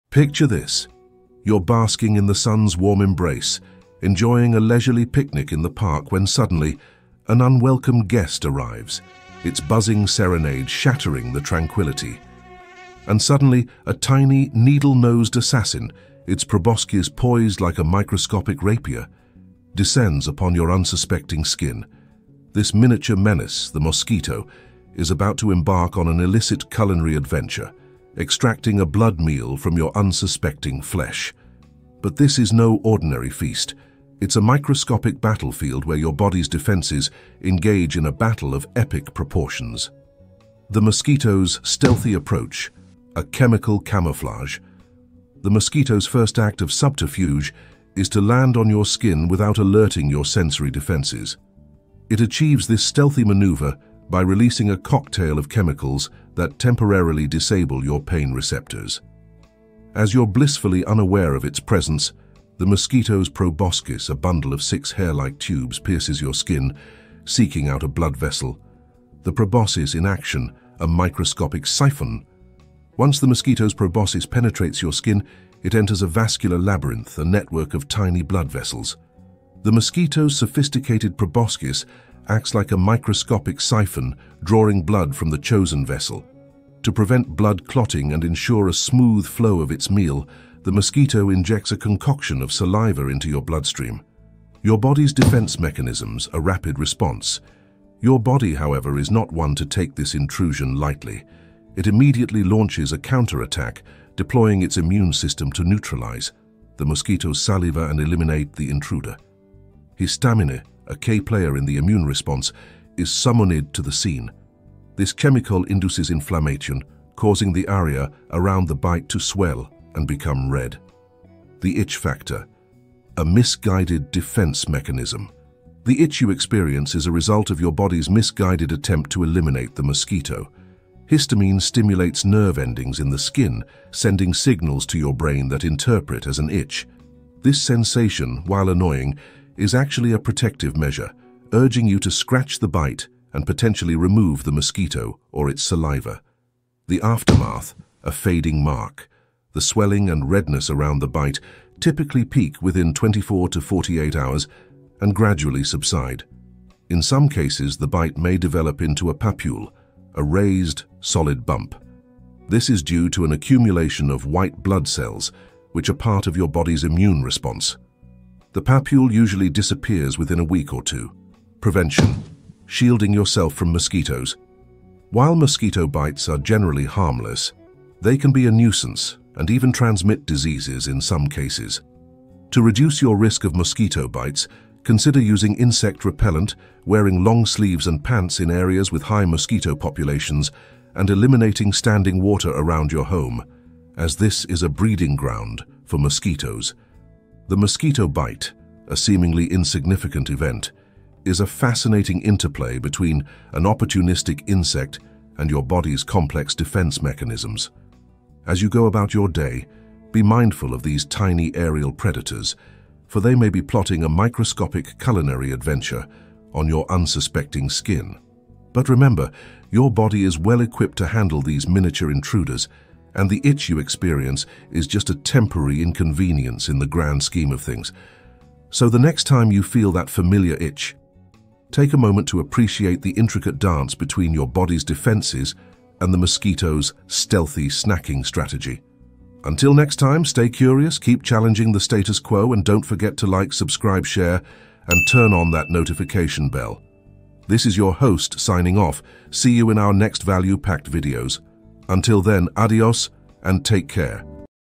🕷️🤯 OMG This is what happens to your body when a mosquito bites you 🫢
Did you know that a mosquito bite can have some interesting effects on your body? In this video, we'll show you exactly what happens when a mosquito bites you and why. From the types of insects that cause bites to the reason why they itch, you'll learn all about mosquito bites and how to prevent them. Watch now and discover some fascinating facts about these pesky little bugs!